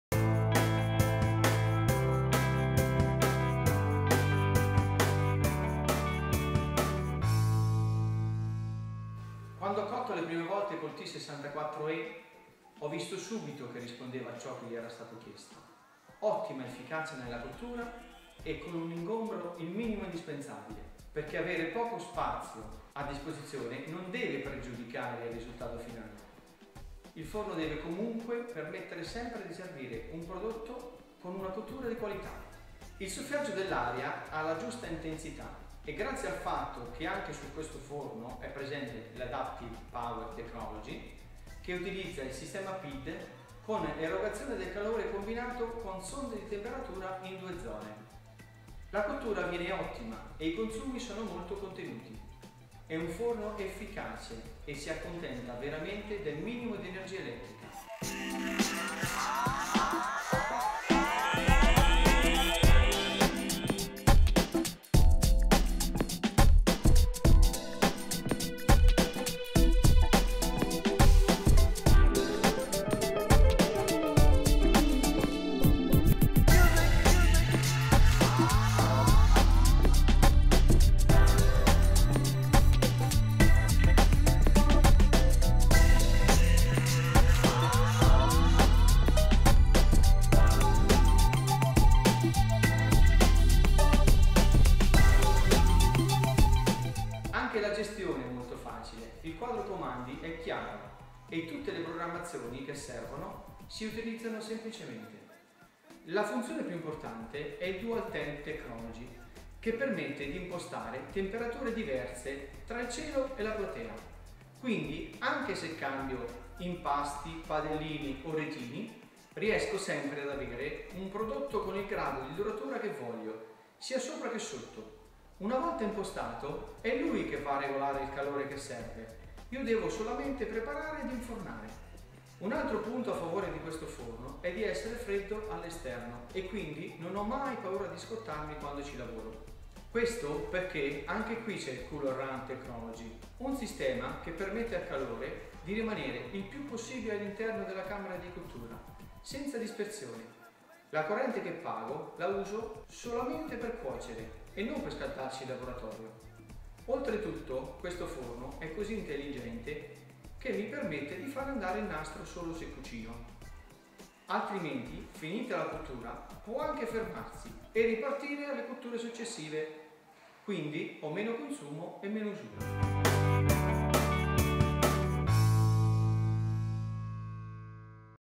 The functions of the T64E - Counter Top Conveyor Oven by Moretti Forni
Demonstrations of the T64E benchtop conveyor oven show just how versatile this compact oven is and how the perfect results are achieved. The oven is perfect for pizzerias, cafes, school canteens, cafeterias, hospitality establishments, catering companies, clubs and hotels.

Euroquip are the exclusive distributors of Moretti Forni in Austrlalia.